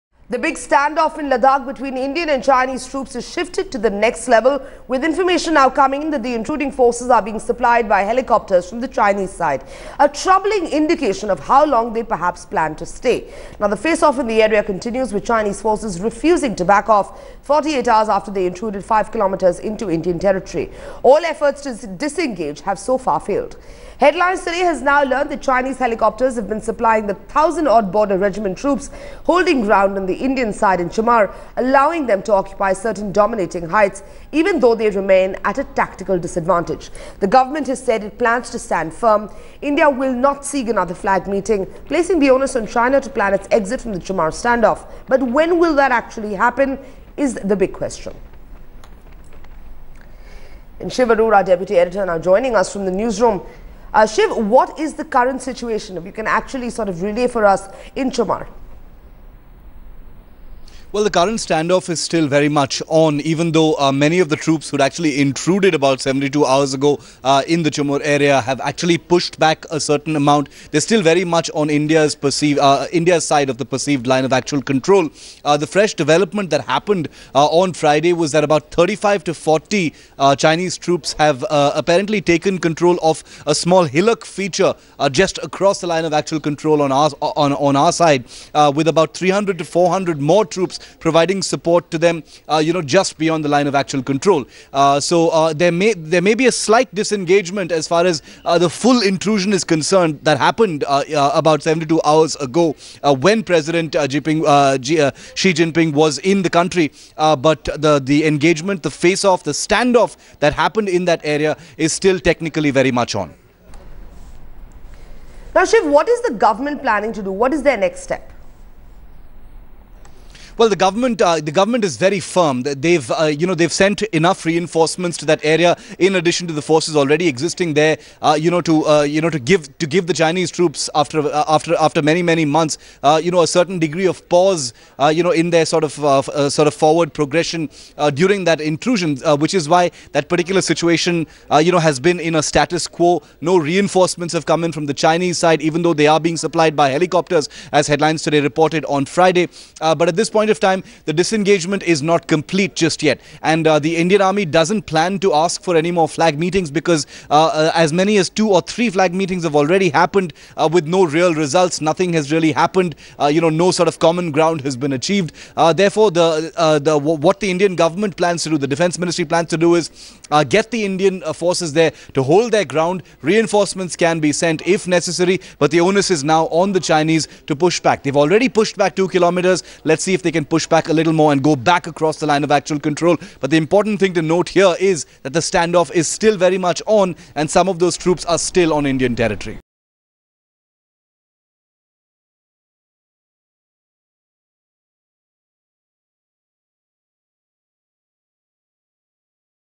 35 PLA personnel perched atop a hillock in Chumar
The big stand off in Ladakh between Indian and Chinese troops have shifted to the next level. The intruding forces are now being supplied by helicopters from the Chinese side. Around 35 PLA personnel has been perched atop a hillock in Chumar. To know more, have a look in.